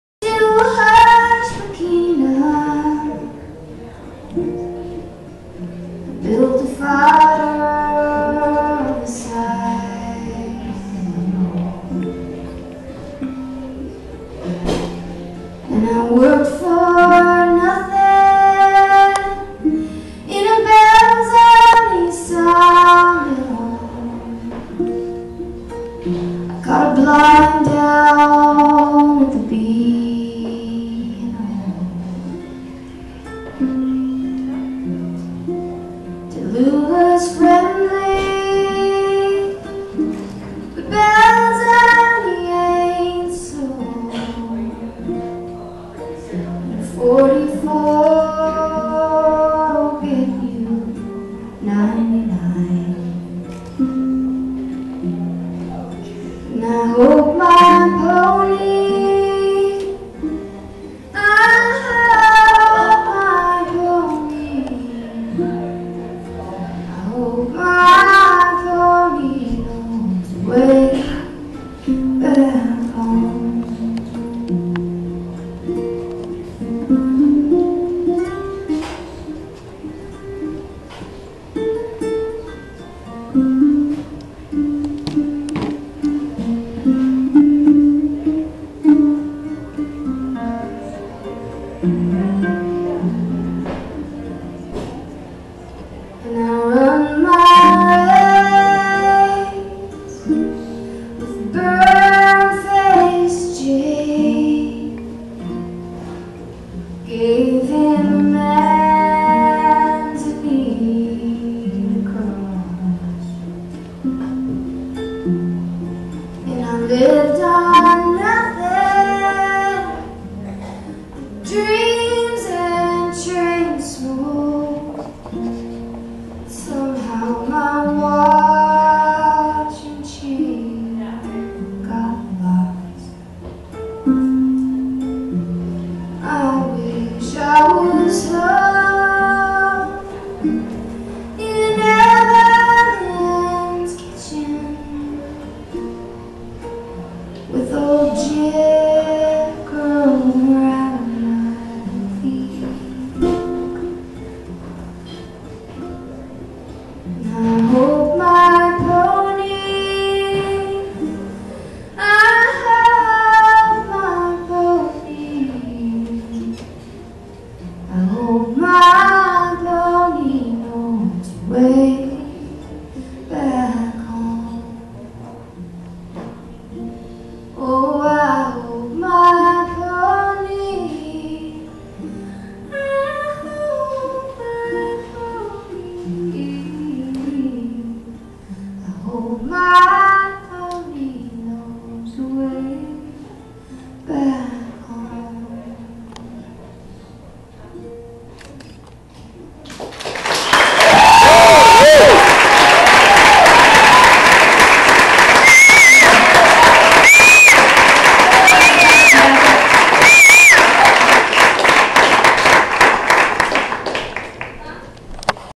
Sara Watkins at Abbey pub 2
Sara Watkins at Abbey pub. This video doesn't do her justice.